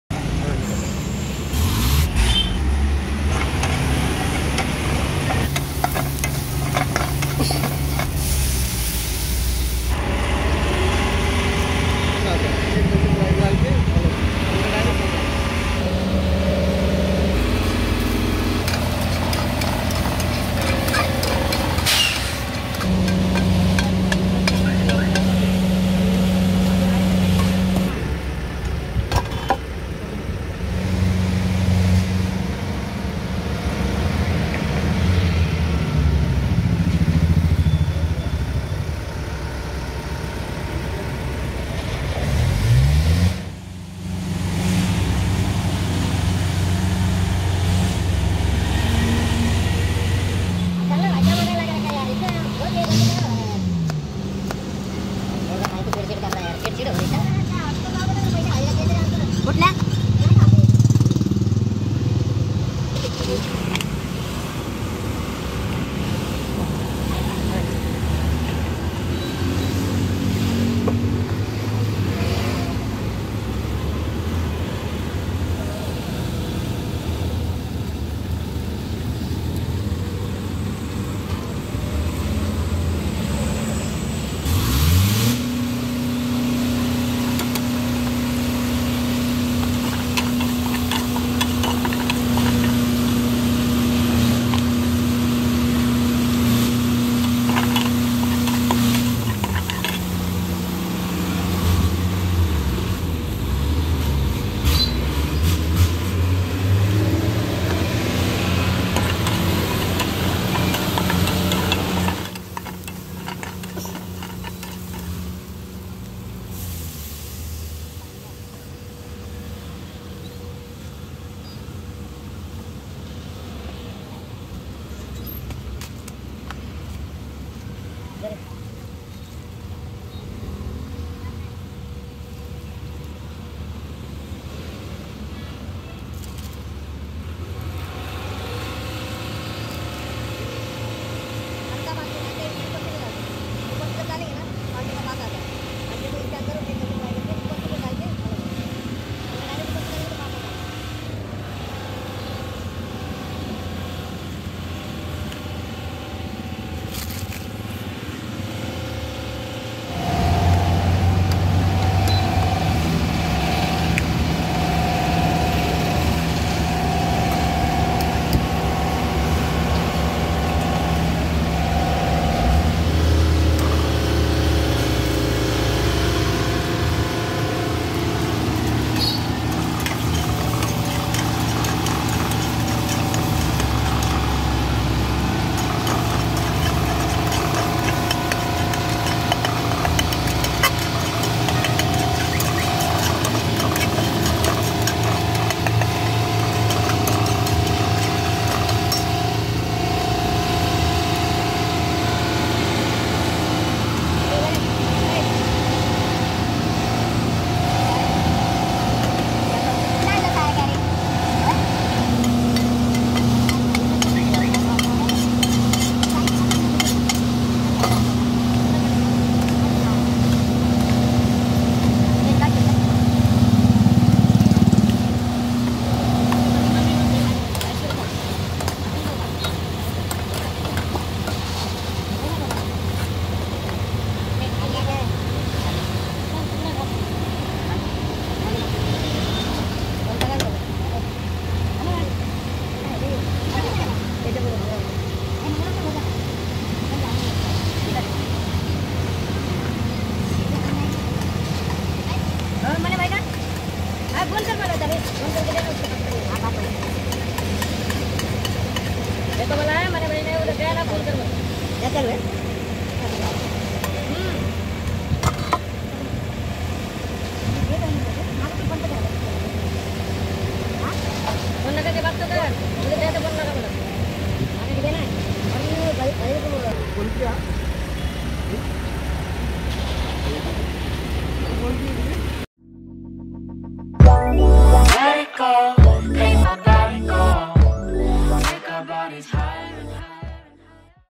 HARDWORKING YOUNG FRIENDS MAKING AMAZING EGG MAGGI | FAMOUS MAGGI WALA OF PUNE | Indian Street Food
Right opposite Symbiosis Pune there is a small joint run by 3 young friends, serving some amazing Maggi.

Street food which is not boring and in-depth

Mumbai tube always try to make a good video.

Price information: Rs 40/-

Location information: Aslam Maggi Corner,
                                       Near Pune Airport, Symbiosis Lane, Pune.